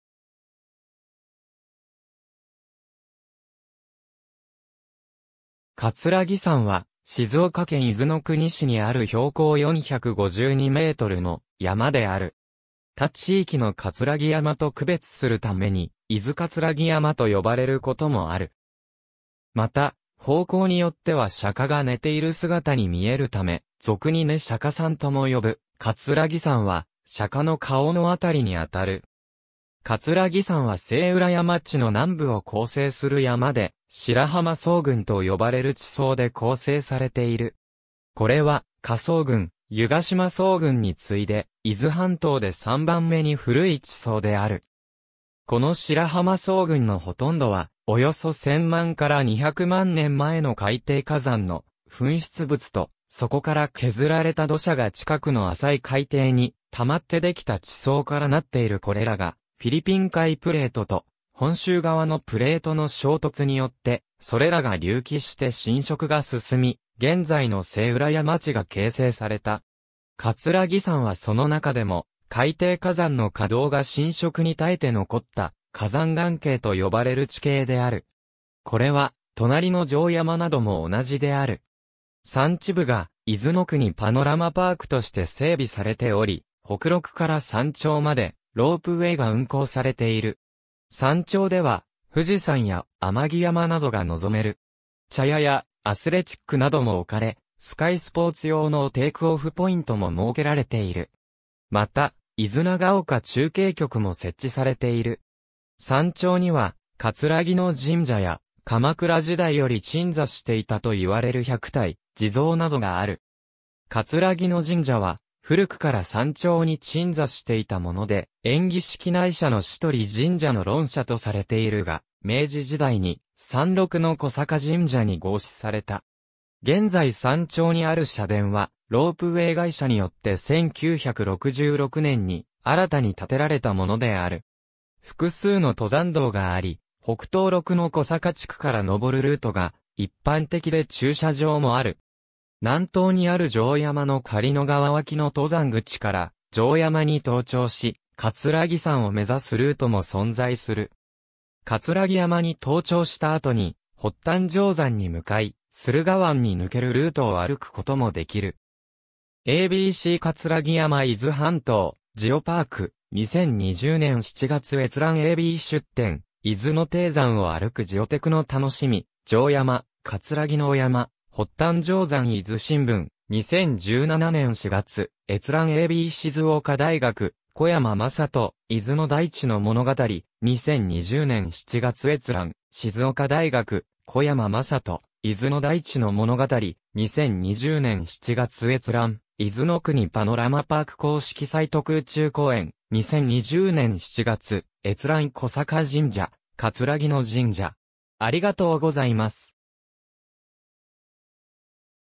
葛城山 (伊豆の国市)
葛城山（かつらぎやま）は、静岡県伊豆の国市にある標高452mの山である。
他地域の葛城山と区別するために伊豆葛城山と呼ばれることもある。
また、方向によっては釈迦が寝ている姿に見えるため、俗に寝釈迦山とも呼ぶ、葛城山は釈迦の顔の辺りにあたる。
葛城山は静浦山地の南部を構成する山で、白浜層群と呼ばれる地層で構成されている。
これは科層群・湯ヶ島層群に次いで、伊豆半島で3番目に古い地層である。
この白浜層群のほとんどは、およそ1千万〜200万年前の海底火山の噴出物と、そこから削られた土砂が近くの浅い海底にたまってできた地層からなっている これらがフィリピン海プレートと本州側のプレートの衝突によってそれらが隆起して侵食が進み、現在の静浦山地が形成された。
葛城山はその中でも海底火山の火道（マグマの通り道にあたる部分）が浸食に耐えて残った火山岩頸と呼ばれる地形である。
これは隣の城山なども同じである。
山地部が伊豆の国パノラマパークとして整備されており、北麓から山頂までロープウェイが運行されている。
山頂では富士山や天城山などが望める。
茶屋やアスレチックなども置かれ、スカイスポーツ用のテイクオフポイントも設けられている。
また、伊豆長岡中継局も設置されている。
山頂には葛城神社や、鎌倉時代より鎮座していたと言われる百体地蔵などがある。
葛城神社は古くから山頂に鎮座していたもので、延喜式内社の倭文（しどり）神社の論社とされているが、明治時代に山麓の小坂神社に合祀された。
現在山頂にある社殿はロープウェイ会社によって1966年に新たに建てられたものである。
複数の登山道があり、北東麓の小坂地区から登るルートが一般的で駐車場もある。
南東にある城山の狩野川脇の登山口から城山に登頂し、葛城山を目指すルートも存在する。
葛城山に登頂した後に発端丈山に向かい、駿河湾に抜けるルートを歩くこともできる。
^ a b c 葛城山 - 伊豆半島ジオパーク、2020年7月閲覧 ^ a b 出典: 伊豆の低山を歩く ジオてくの楽しみ(2) 城山・葛城山・発端丈山 - 伊豆新聞、2017年4月閲覧 ^ a b 静岡大学 小山真人 「伊豆の大地の物語」（12）、2020年7月閲覧 ^ 静岡大学 小山真人 「伊豆の大地の物語」（78）、2020年7月閲覧 ^ 伊豆の国パノラマパーク公式サイト 空中公園、2020年7月閲覧 ^ 小坂神社（玄松子）;葛城神社（延喜式神社の調査）